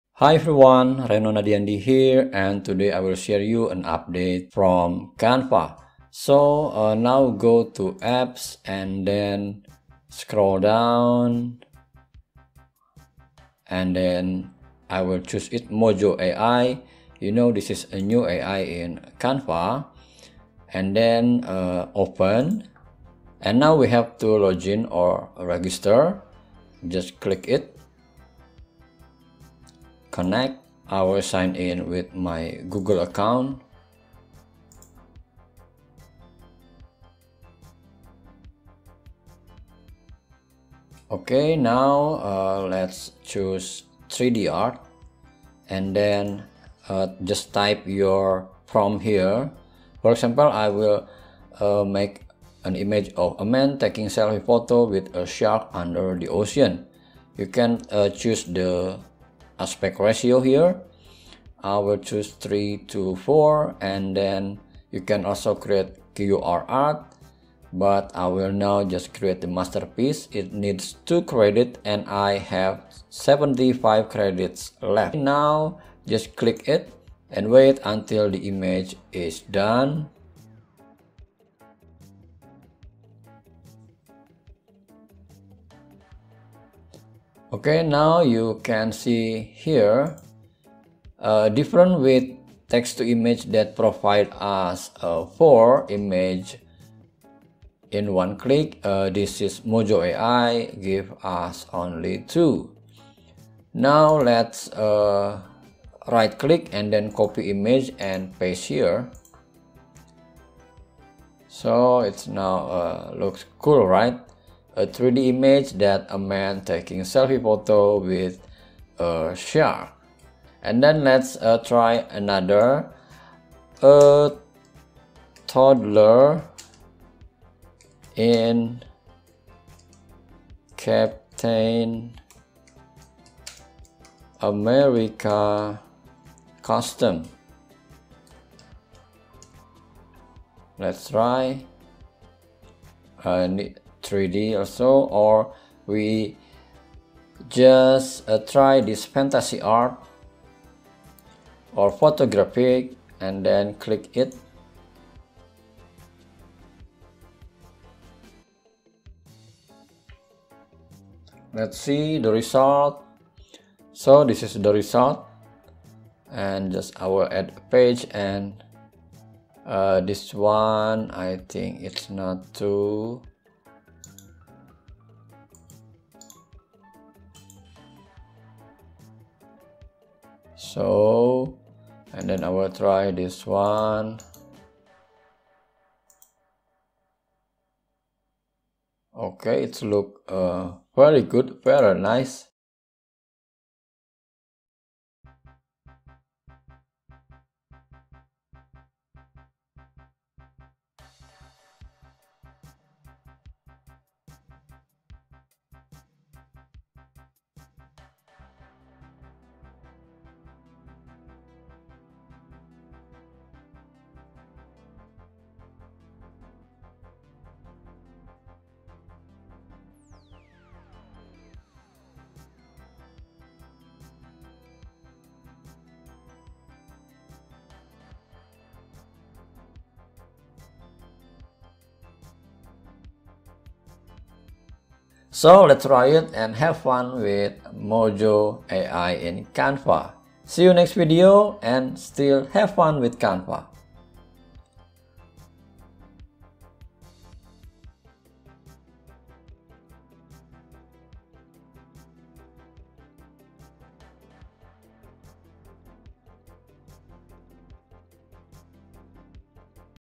New Image Generator in Canva | Create 3D Image Using AI in Canva Tutorial
In this video, I'll show you how to use Canva's new image generator AI to create awesome 3D images.

Welcome to Canva Art Design with Reno Nadiandy.
today i will share you a Canva Update about their new AI for image generating. This is Mojo AI.

This feature is perfect for creating eye-catching graphics for your social media posts, website, or print materials. I'll walk you through the steps from start to finish, so you can start creating your own 3D images in no time.

Here are some additional tips for creating great 3D images with Canva:

Use high-quality images as your base.
Experiment with different styles and effects.
Don't be afraid to get creative!

Image generator AI,3D images